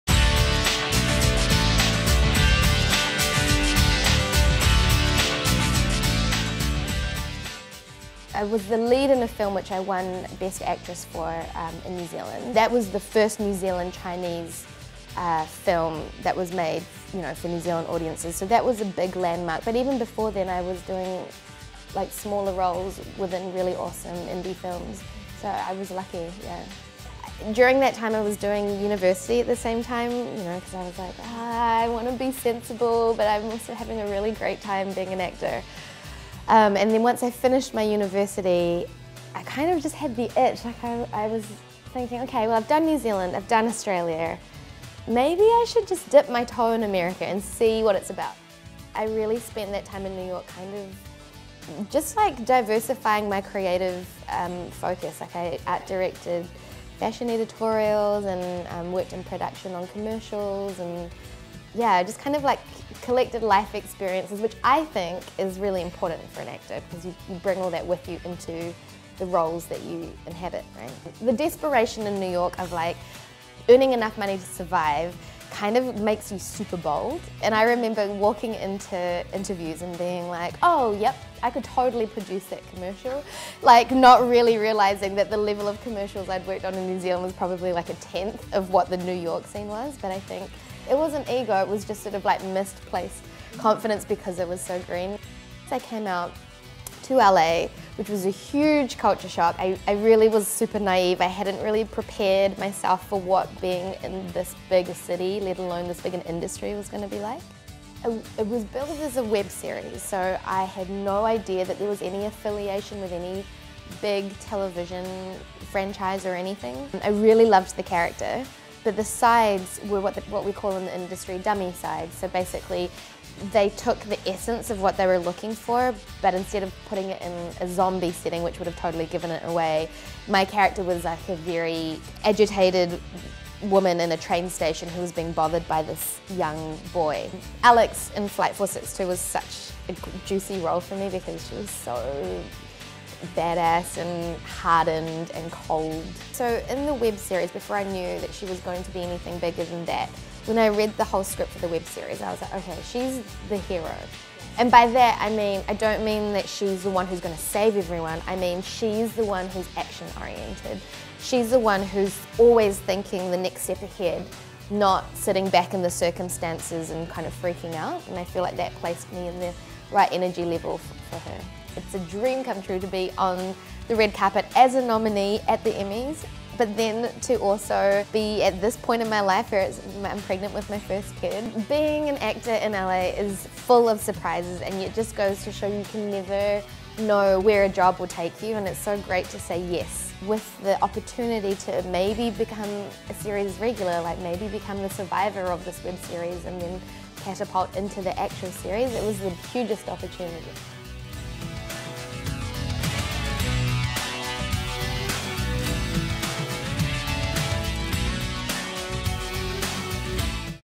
Meet Michelle Ang, the Emmy-nominated actress of 'Fear the Walking Dead'
Michelle Ang, the New Zealand actress making waves in Hollywood, talks beginnings and the show - "Fear the Walking Dead" - that got her an Emmy nomination.

Host: Emily Tang
Cinematography: Wesley Samilin, Stacey Wong
Audio: Charles Gray
Producer: Eric Nguyen